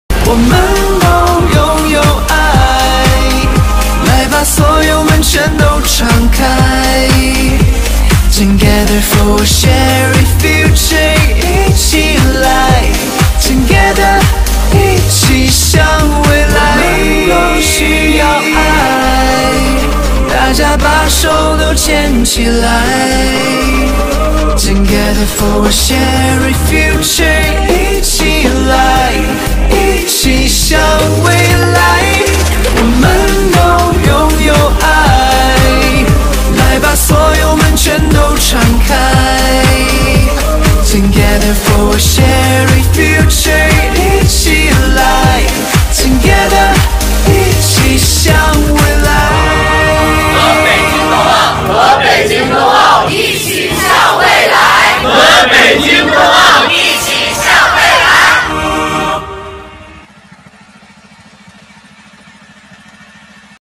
7HP vertical axle mini power tiller work in Banana Orchards
We must look to the future.
7HP gasoline vertical-axle farmmachinery powertiller minipowertiller 多功能微耕机 work in bananaorchards videos share with you.